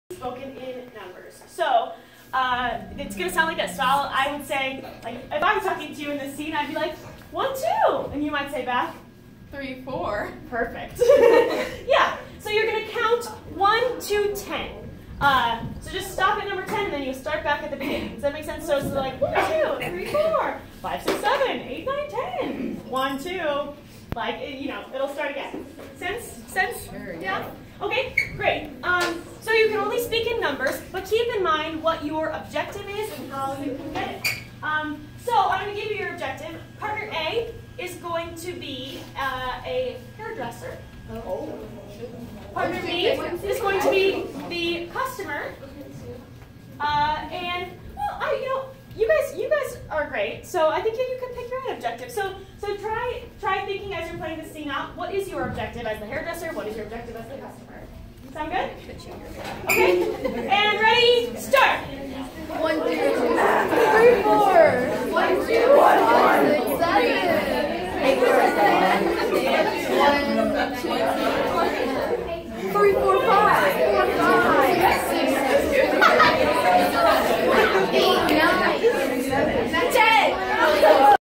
Scene work talking in numbers 1-10
Drama exercise to develop vocal expression and gestures